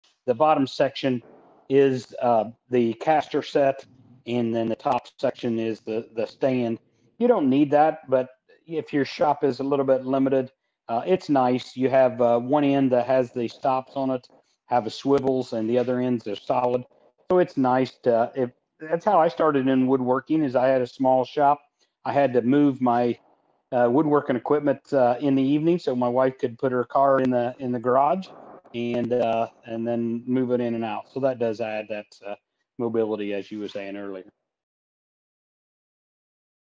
Get your CNC on Wheels
Laney and Burl talk about the castor set for the bottom of your DWC1824 Mini Carver work stand. This makes it easy to share the space with other machines or even your car in the garage.